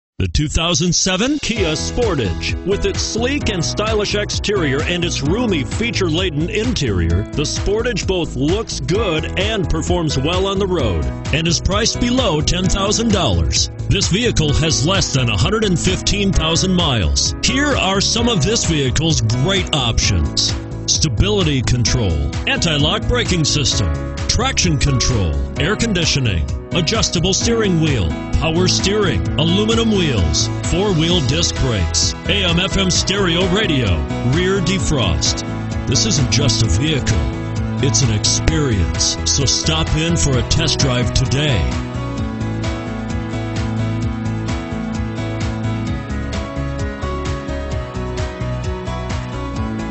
2007 KIA SPORTAGE Bismarck, ND 222219
2007 KIA SPORTAGE Bismarck, ND
Stock# 222219

 1 OWNER that is CLEAN and features aluminum wheels, ice cold air conditioning, power equipment, and is an excellent 4x4 under $10,000! Stop in and see this one before it's too late!